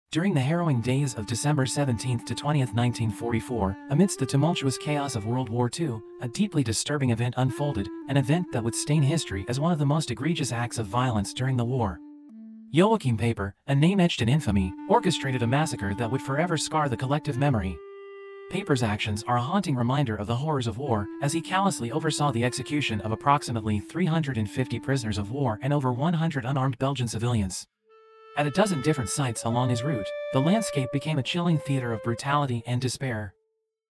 The Darkest Days of WWII The Massacre of POWs and Civilians 🌍😢WWIIMassacre
During the harrowing days of December 17-20, 1944, amidst the tumultuous chaos of World War II, a deeply disturbing event unfolded – an event that would stain history as one of the most egregious acts of violence during the war. Joachim Peiper, a name etched in infamy, orchestrated a massacre that would forever scar the collective memory.

Peiper's actions are a haunting reminder of the horrors of war, as he callously oversaw the execution of approximately 350 prisoners of war and over 100 unarmed Belgian civilians. At a dozen different sites along his route, the landscape became a chilling theater of brutality and despair.

One particularly horrifying incident occurred at the Baugnez crossroads near Malmedy. On that fateful day, Peiper's armored units and half-tracks came face to face with a convoy of around thirty American vehicles, their occupants woefully outmatched. What followed was a senseless bloodbath, as Peiper's forces gunned down these defenseless prisoners of war and innocent civilians.

This episode serves as a grim testament to the darkest facets of human nature during times of conflict. The haunting echoes of those tragic days continue to resonate through history, a stark reminder of the need to strive for peace, compassion, and the preservation of human dignity.

As we reflect upon this somber chapter, let us honor the memory of those who fell victim to this heinous act and renew our commitment to a world where such atrocities are never repeated. 🌍🕊️